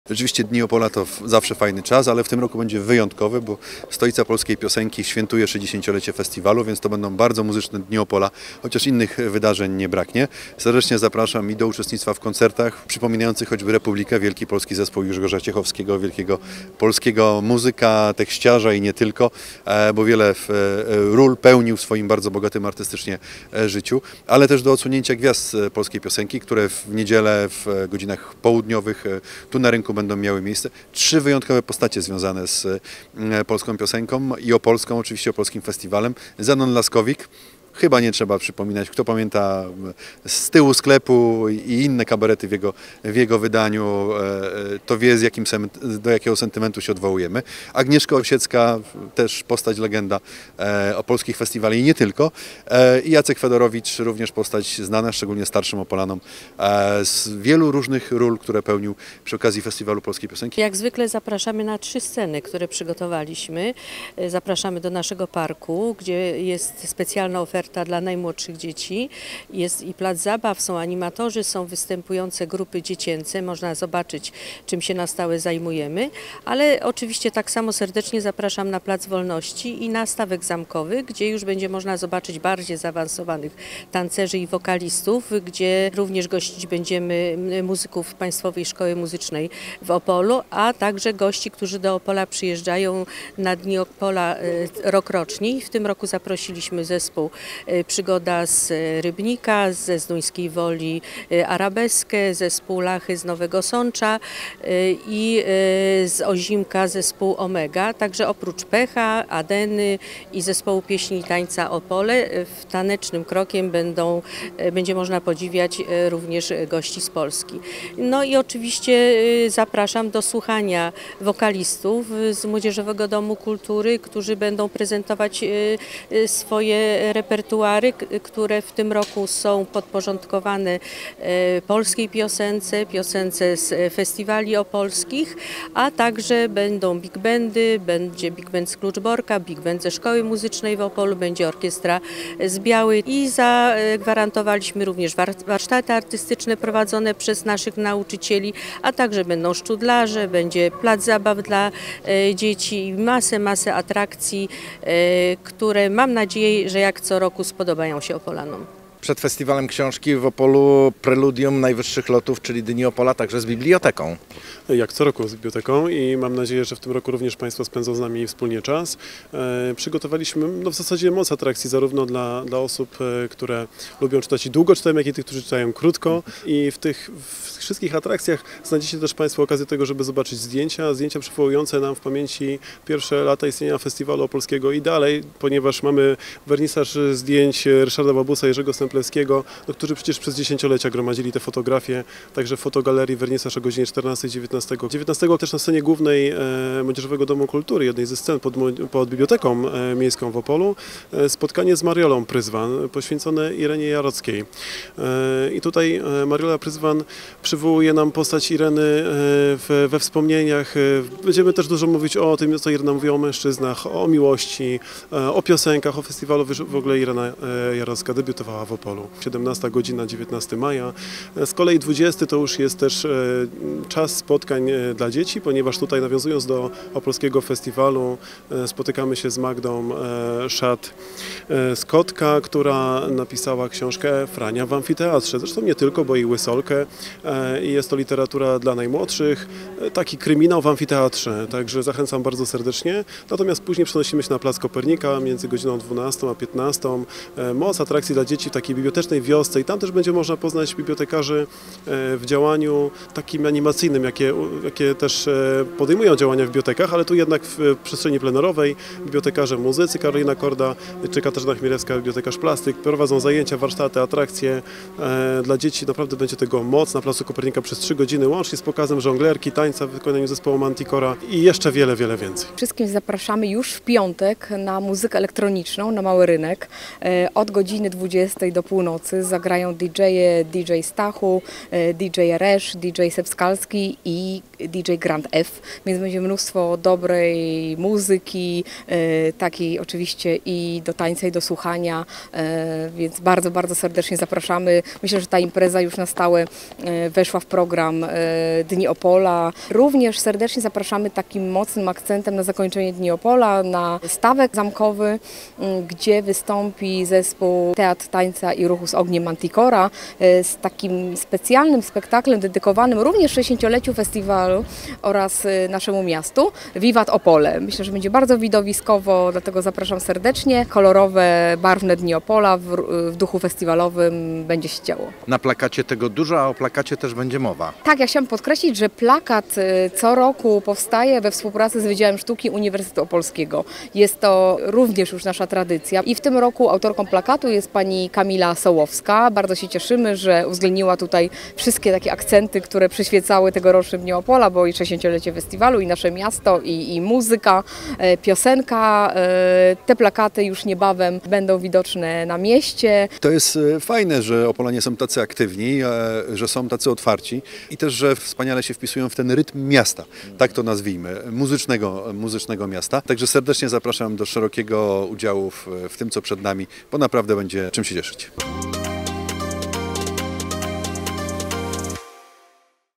Dni Opola 2023 - zapowiedź
Trzy dni igrców, zabaw, koncertów i mnóstwa wydarzeń o charakterze kulturalnym i rekreacyjno-sportowym znajdzie się w programie tegorocznych Dni Opola. Na święto miasta i jego mieszkańców zapraszamy w dniach 19-21 maja 2023.